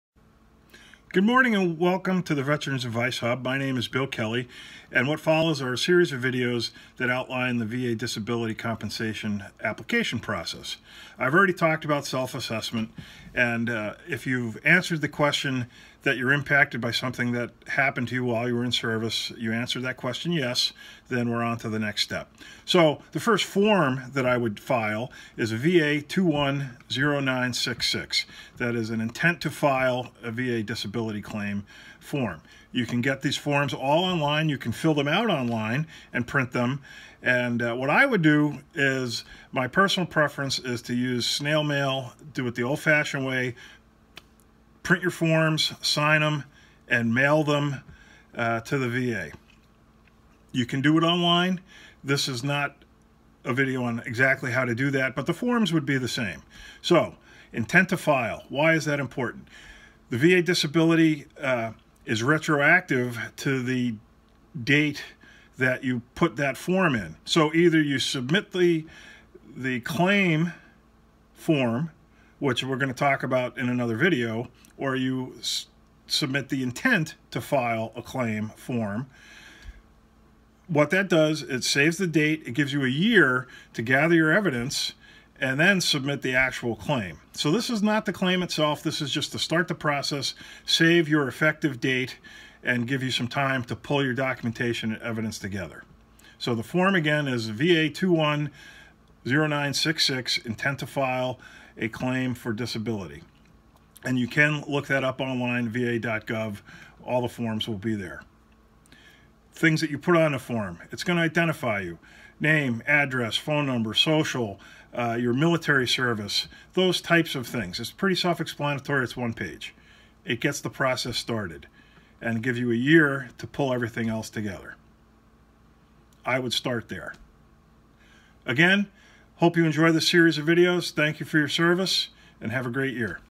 VA Claim Process - VA21-0966 Intent to File
VA21-0966 Intent to File a Claim – this serves to inform the VA you will be filing a claim within 365 days and establishes the effective date of that claim back to the date the VA processed this form.  People send this in first to get the effective date established, then dig in to gathering documentary and medical evidence.

Here is the mailing address for claims:
DEPARTMENT OF VETERANS AFFAIRS
CLAIMS INTAKE CENTER

This is a one page form.

Section I: General Benefit Election
You are intending to file for "Compensation" not a Pension (pensions are a whole other process).

Section II: Claimants Identification
Name, Social Security Number, Date of Birth, Address, Phone, Email, etc.

Section III: Declaration of intent
Read, sign and date.  If you filed a VA 21-22 Appointment of Veterans Service organization as Claimants Representative form (next video), they can sign for you.